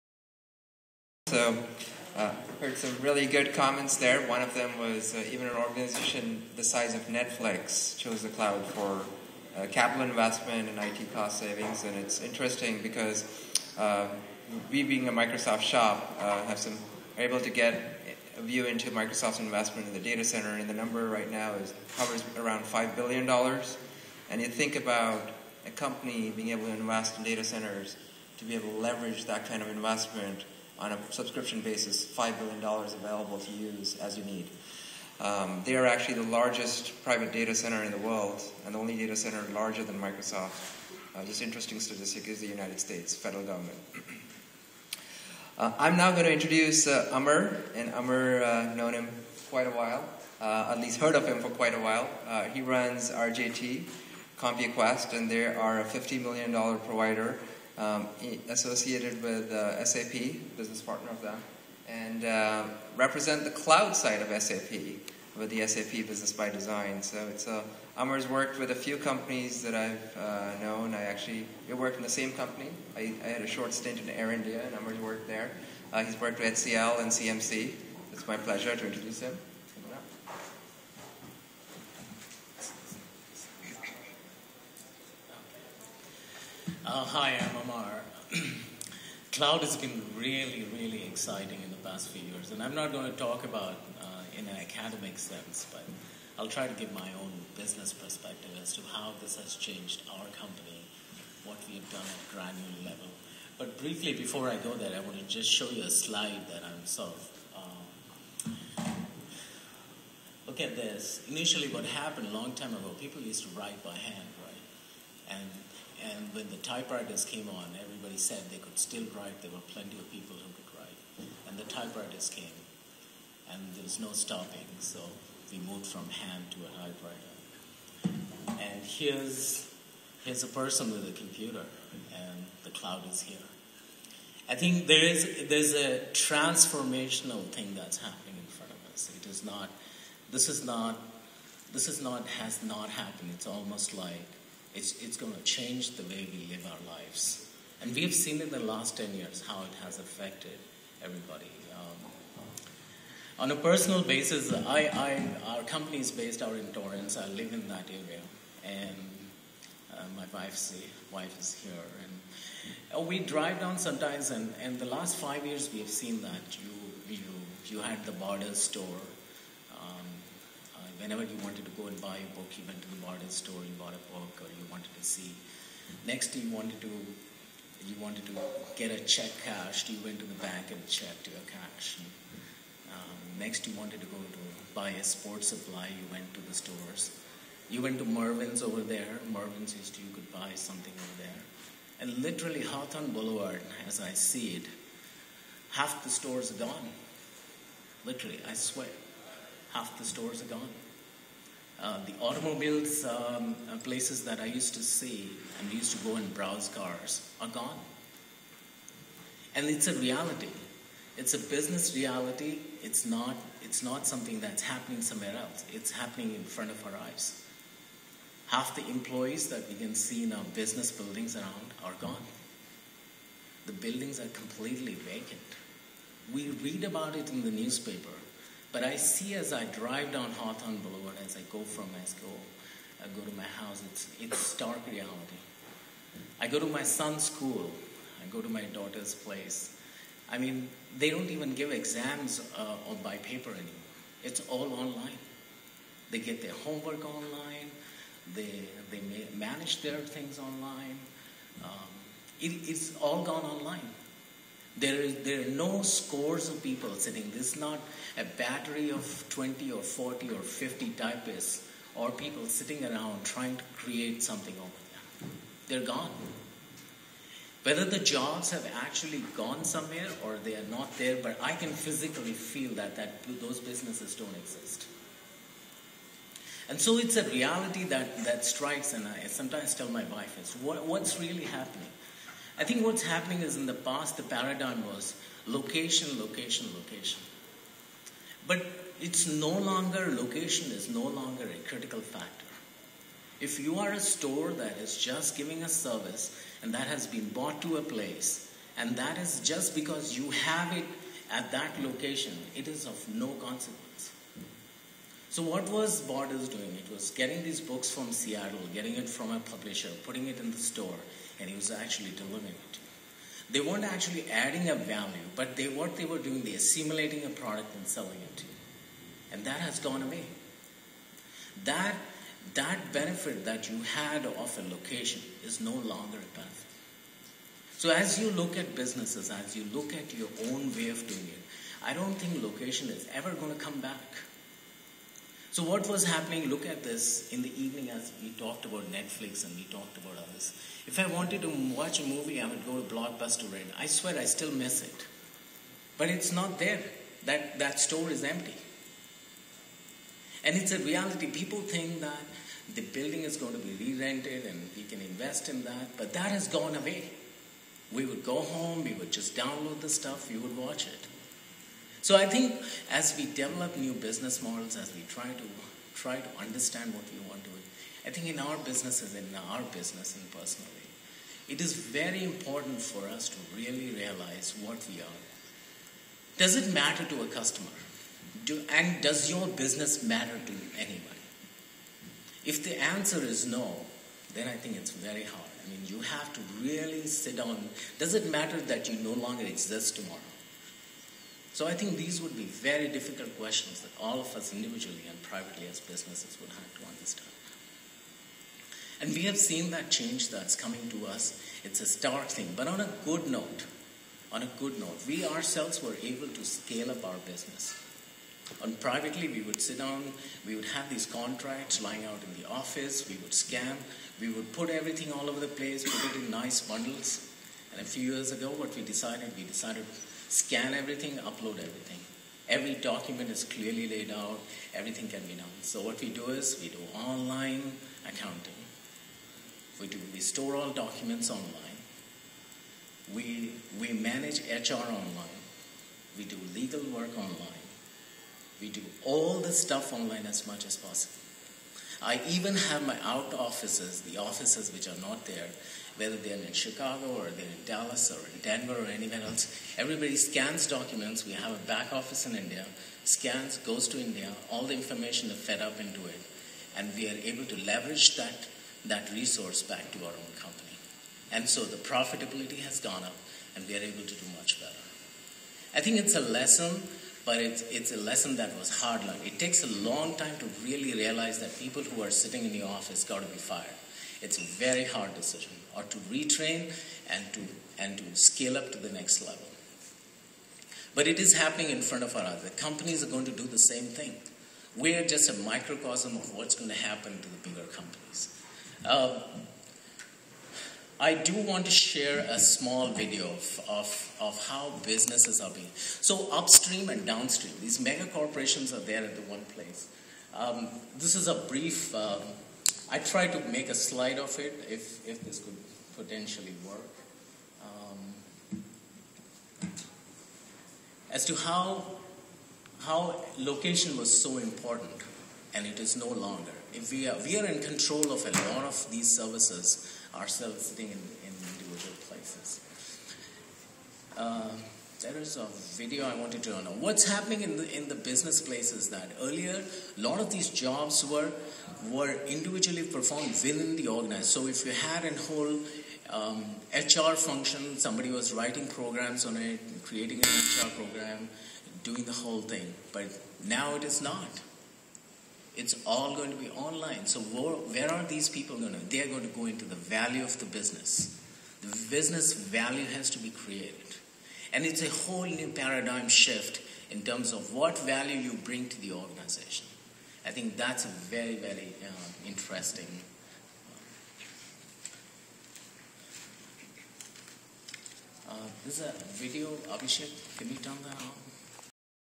Amar Shokeen on Cloud Computing
Amar shares his insghts and experiences with Cloud Computing and offers a few keen observations about the future of business with Cloud Computing at TieCON 2011 at Long Beach, CA.